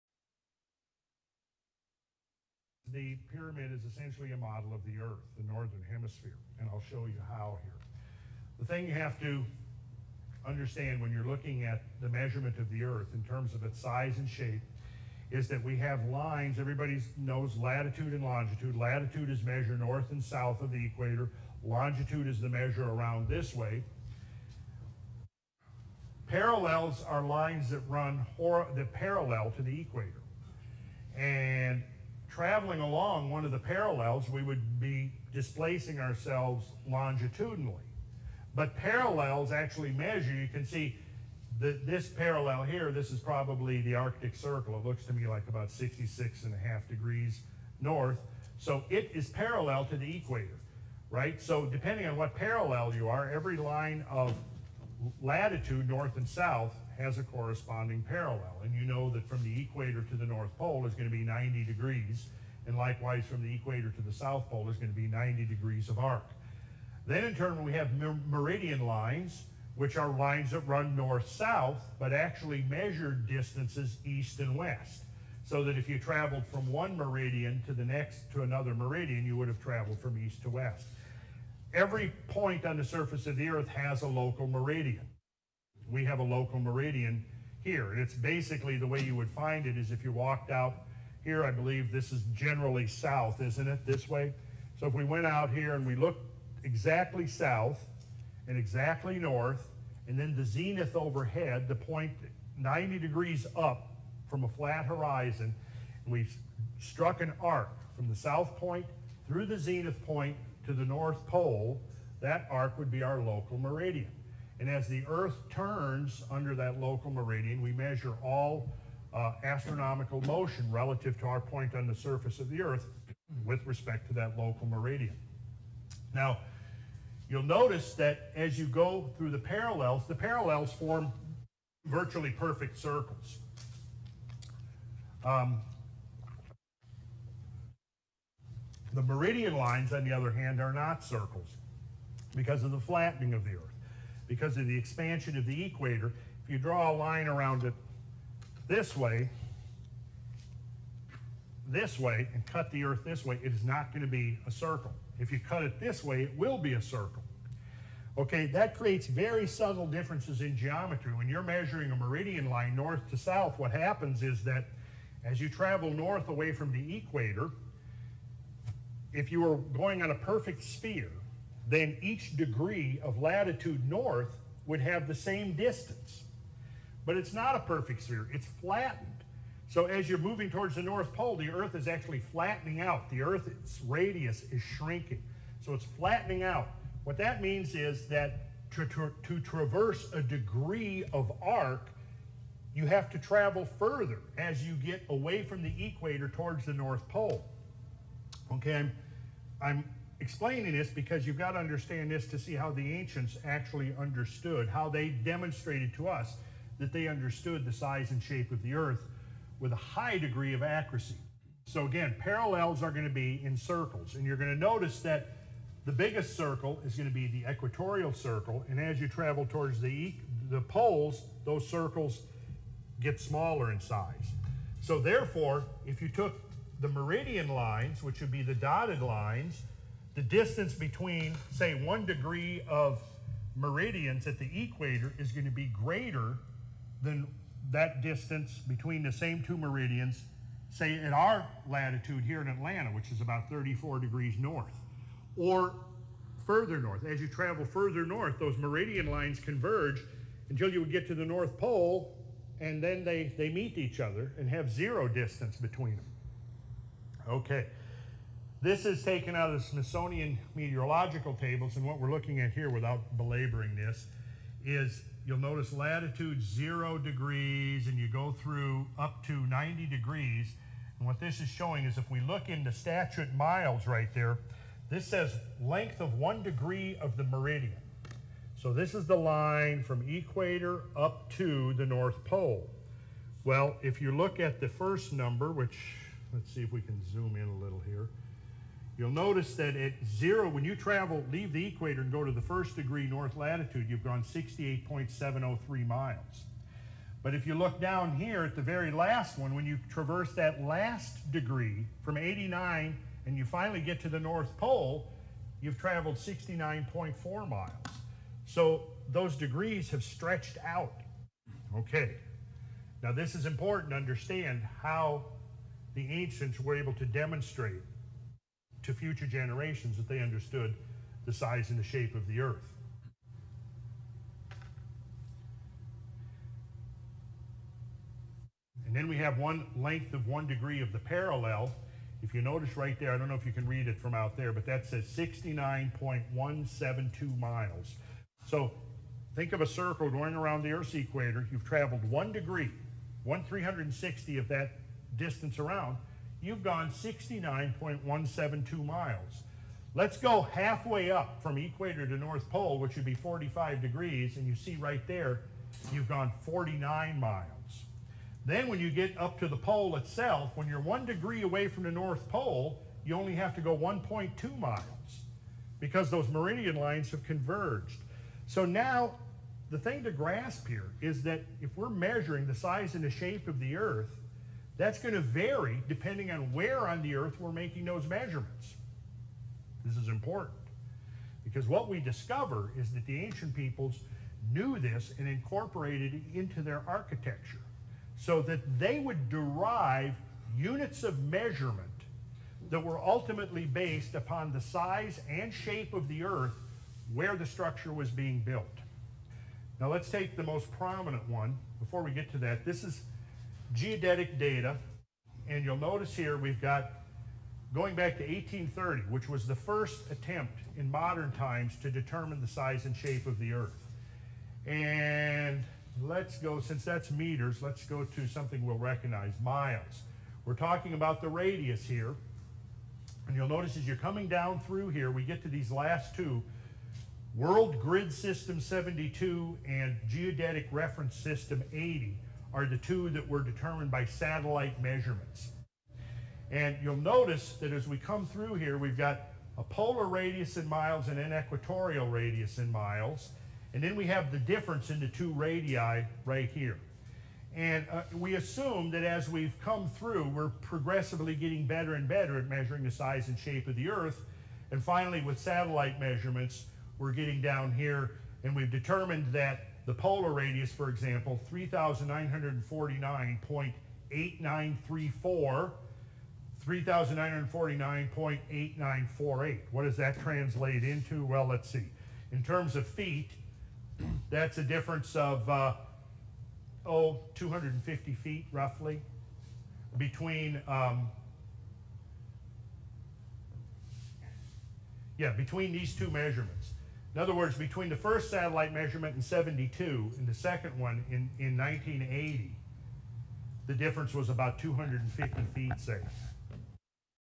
Flat Earth: The Egyptians began the Ball Earth Lie, 100% Proof, part 2
Here we see a professed "MASON" spreading the lies of the Egyptian Masons, pretending that we are living on a spherical Earth with "Flat Poles" LMMFAO! Well at least they got the Flat Part of it right! ha ha ha. Just more proof that those in power have always wanted us to believe in silly lies like a spinning ball for an Earth.  Santa Ballers fall for the psyops of tptb so easy. FAIL!